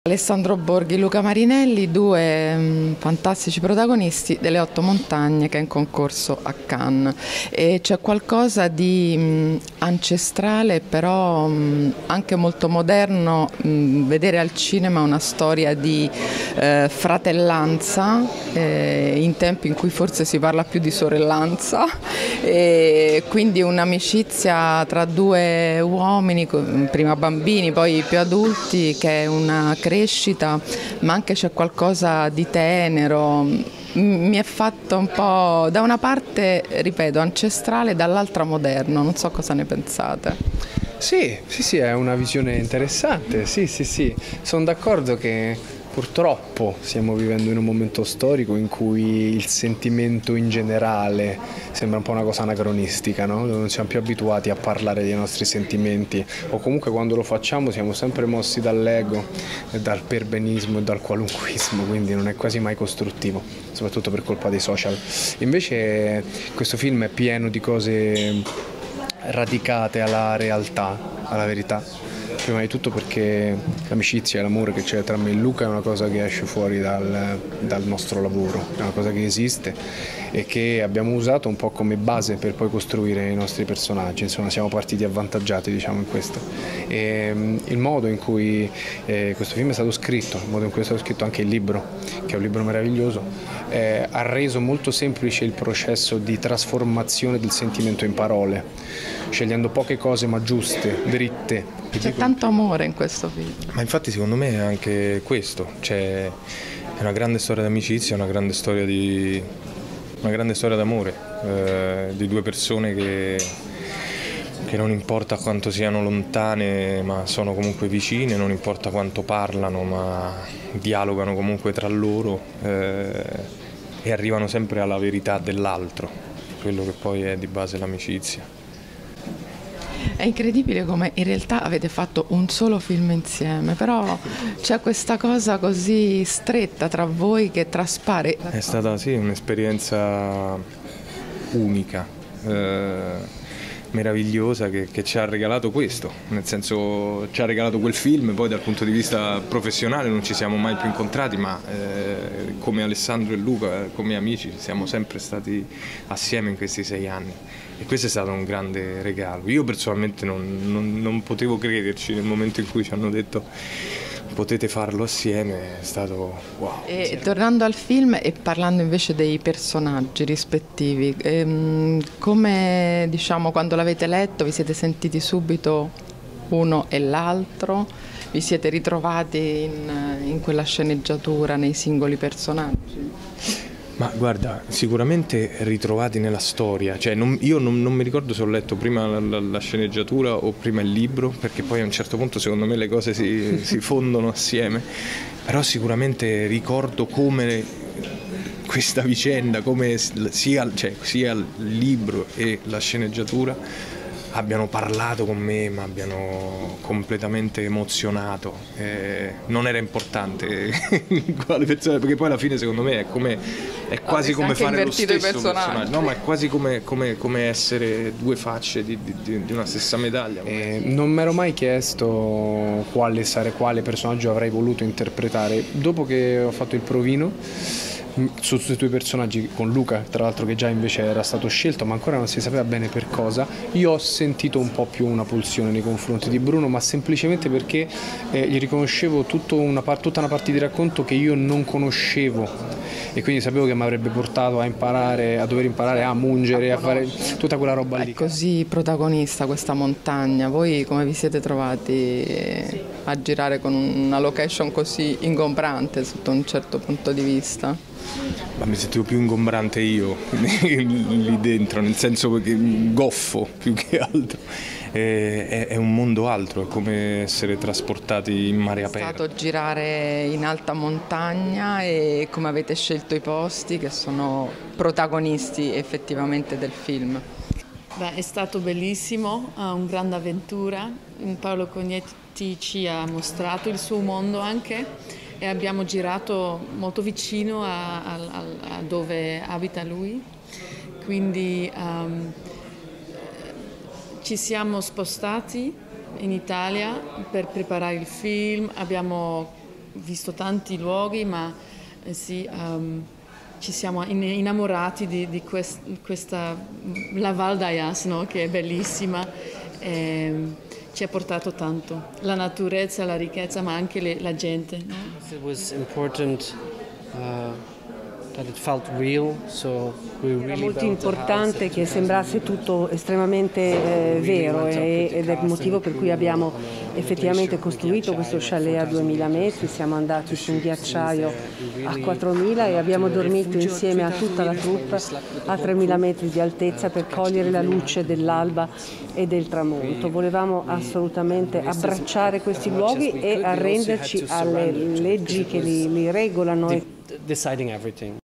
Cannes, "Le otto montagne" di Borghi e Marinelli
"Film sull'amicizia, siamo partiti avvantaggiati". Regia di Vandermeersch e Van Groeningen